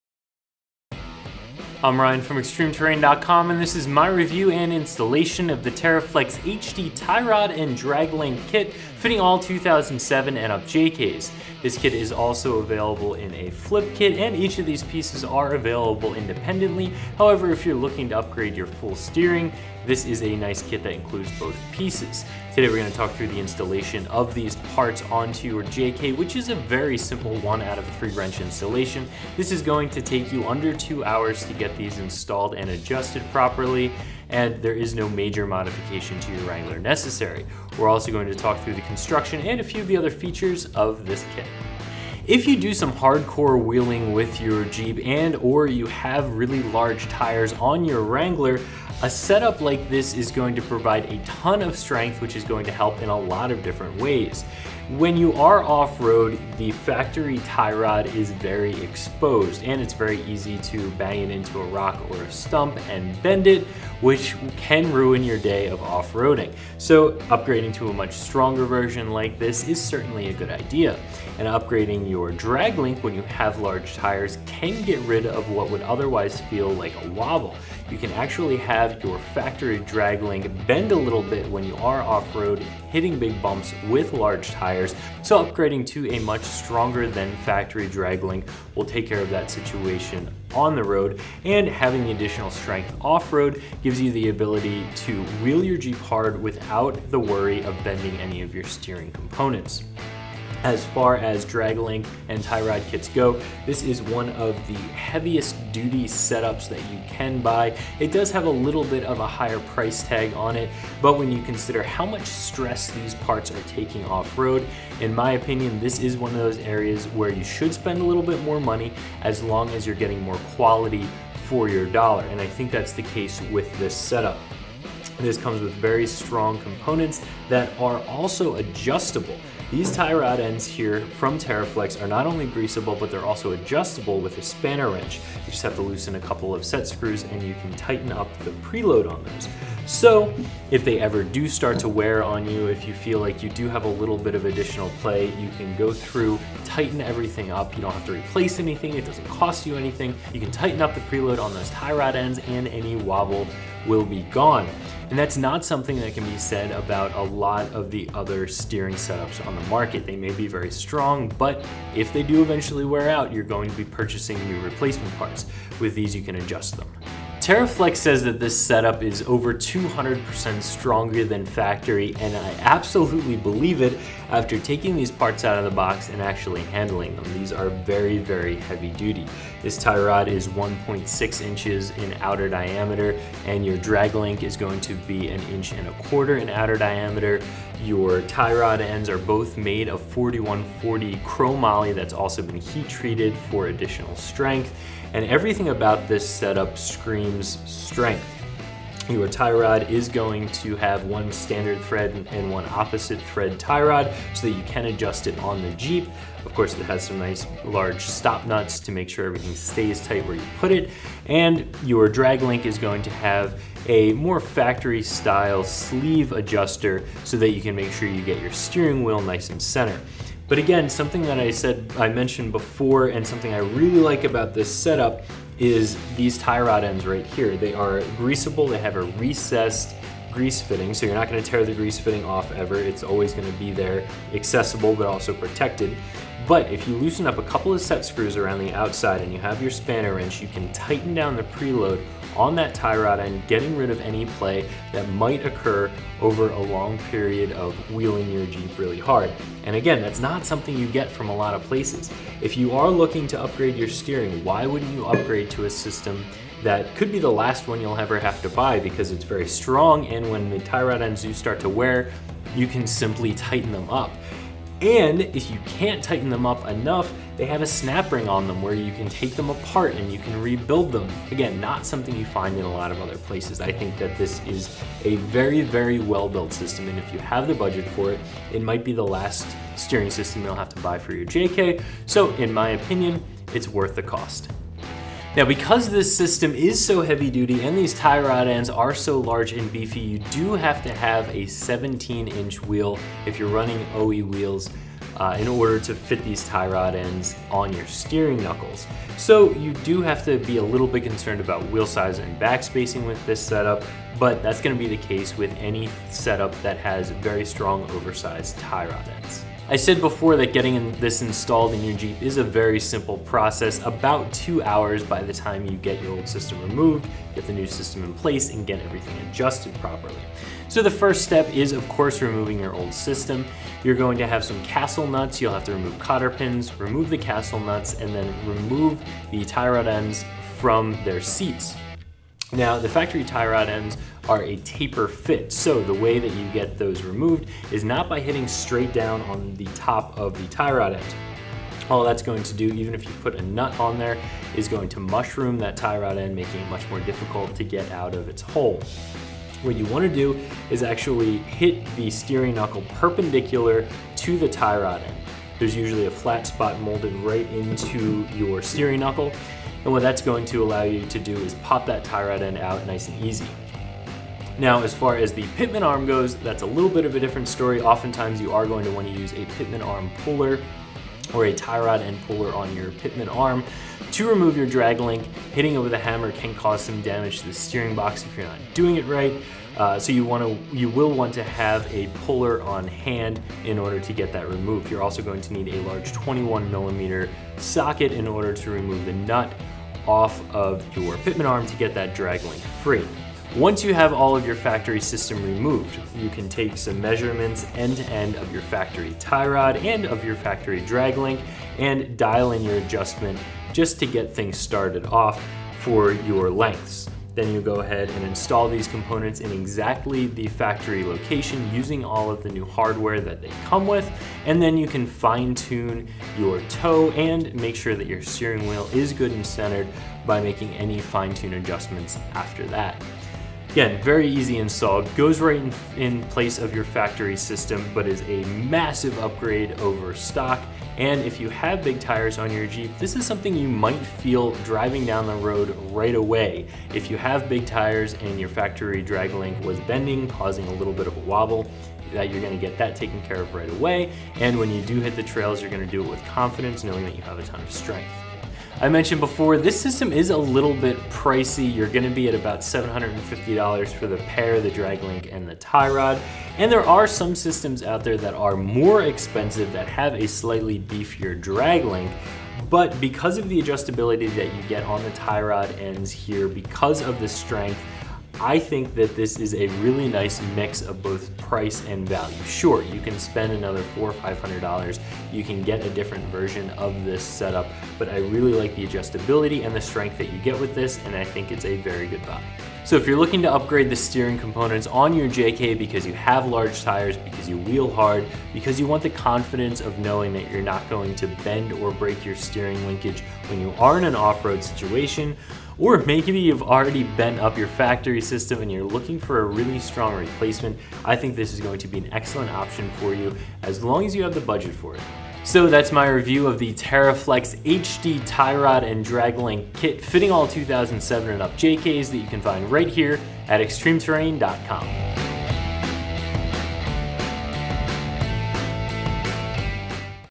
Jeep Wrangler (2007-2017 JK) Teraflex HD Tie Rod & Drag Link Kit Review & Install
Strengthen the steering linkage for intense off-road trails with the Teraflex HD Tie Rod & Drag Link Kit! This kit is adjustable, durable, and high quality to ensure the best possible operation.

Item J103702
MPN# 1853900

More Strength and More Control. Did you know that the factory steering linkage is one of the few weak points on your 2007 to 2017 Wrangler JK? This holds especially true if you are running a lifted suspension set-up with larger off-road wheels and tires. If you like traversing the untamed wilderness, then you can’t go wrong with this Teraflex Tie Rod and Drag Link Kit. This system is more than 200% stronger and more reliable than the stock steering linkage in your rig. This will not only give your Jeep more strength in terms of steering action, but this kit will also offer you more control in any type of inhospitable terrain.

Heavy-Duty Construction. The Teraflex HD Tie Rod and Drag Link Kit is constructed using heavy-duty 1.25-inch forged steel. The tie rods are constructed from 1.6-inch heat-treated Chromoly steel, while the tie rod ends are made from a potent blend of 4140 Chromoly ball and stud with a 1045 carbon steel housing. Adding it all up, this kit is designed to provide many years of reliable performance.

Professional Installation Required. The Teraflex HD Tie Rod and Drag Link Kit is a direct bolt-on replacement for the factory steering linkage in your 2007 to 2017 Wrangler JK. Professional installation is highly recommended due to the complex nature of the install.